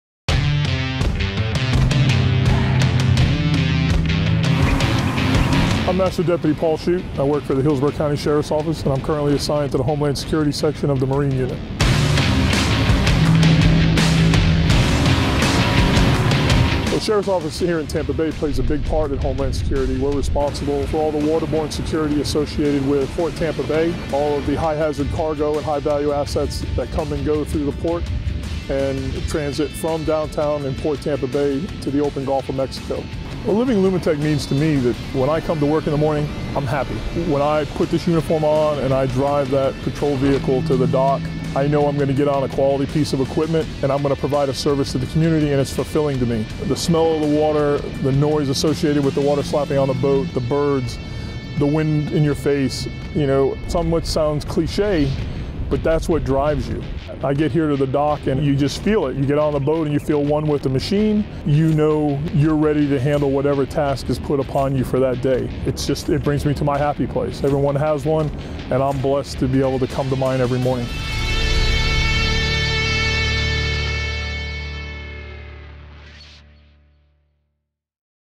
Living Lumitec Safe & Secure
Master Deputy Paul Shute of the Hillsborough County Sheriff's Office shares his insights to what the job means to him. Living Lumitec is a passion for boating and the marine lifestyle whether you work on the water of just enjoy spending all your feel time out there. What does "Living Lumitec" mean to you?